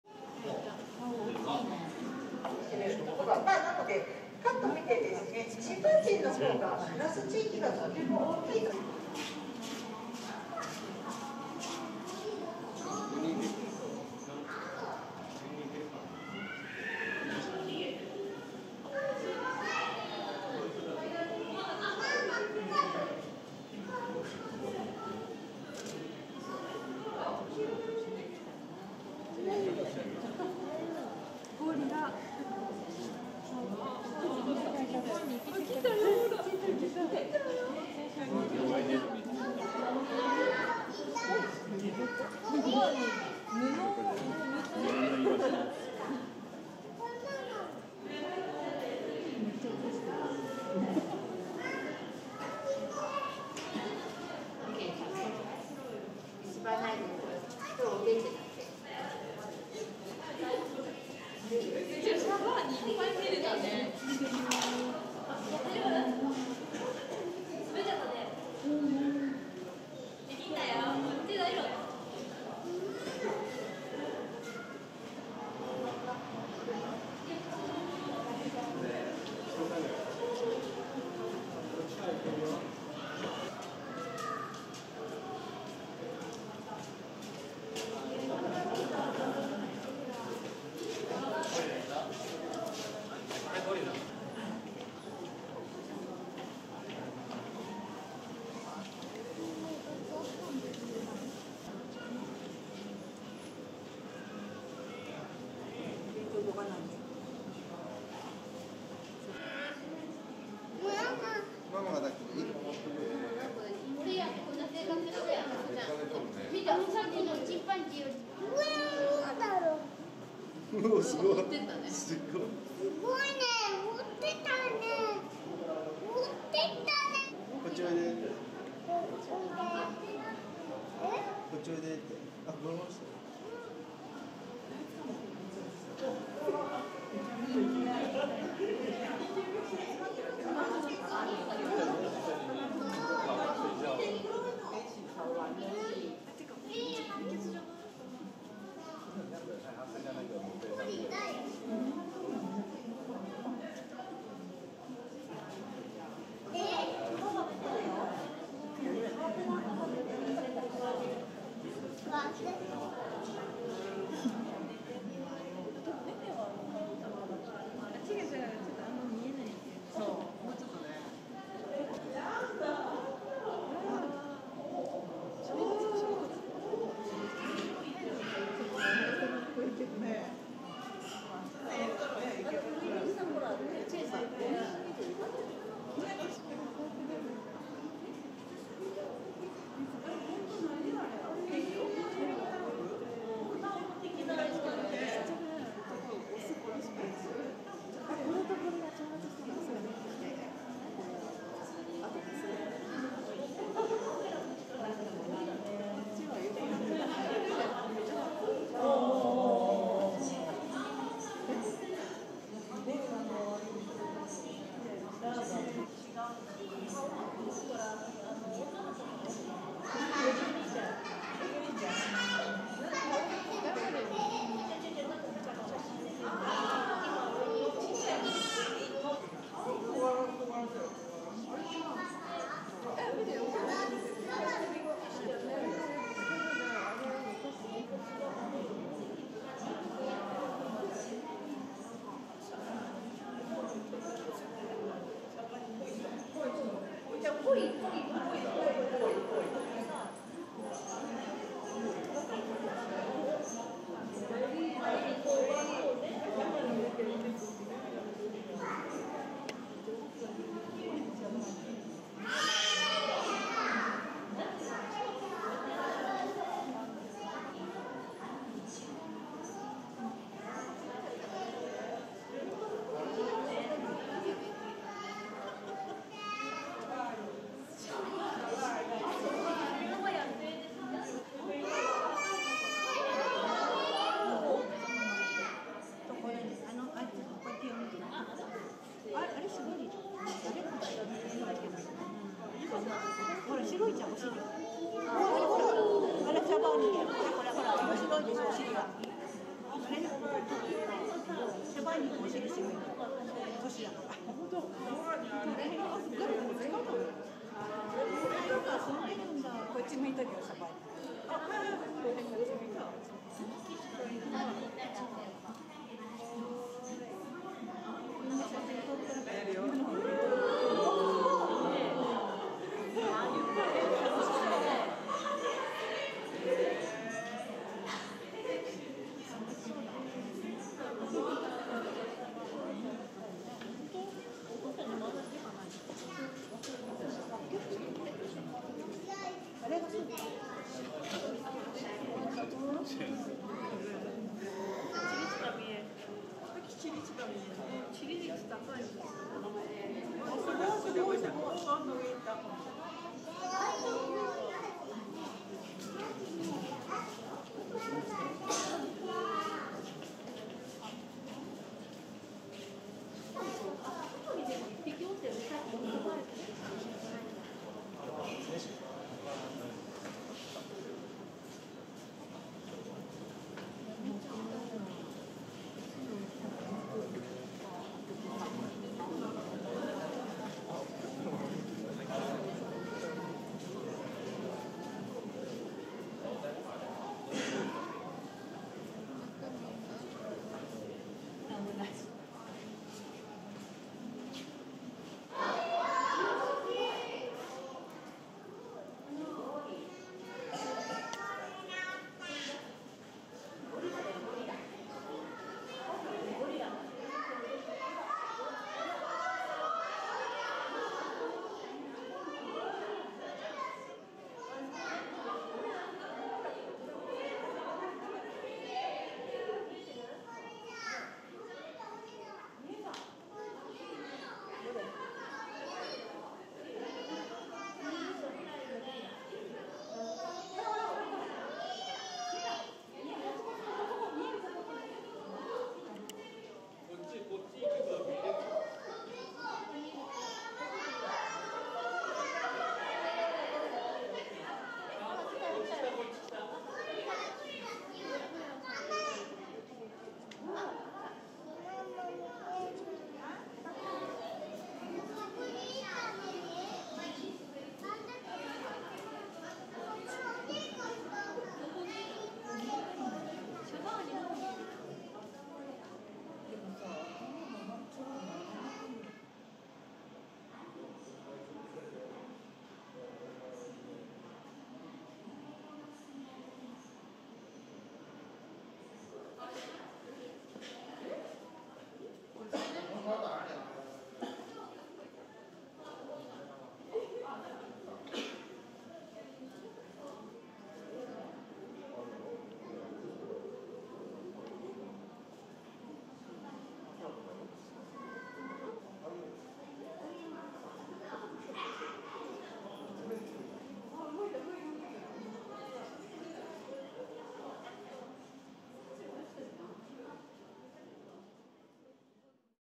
Gorilla Daughter Is Always on Dad's Side | The Shabani Group
Shabani and Kiyomasa fighting over a burlap sack.
Shabani's daughter Annie, who was watching the whole thing, was miffed at Kiyomasa.
She was showing off to Kiyomasa.
Annie is always on her dad Shabani's side.

TIME STAMPS
00:00 Gorillas relaxing indoors
00:16 Shabani and Kiyomasa in a tussle
00:42 Annie
01:36 Kiyomasa takes where Ai is resting
02:40 Annie is miffed at Kiyomasa
03:18 Annie showing off to Kiyomasa

Shabani: Male Born October 20, 1996
Nene: Female Born in 1972 (estimated)
Ai: Female Born February 27, 2003
Kiyomasa: Male Born November 1, 2012
Annie: Female Born June 2, 2013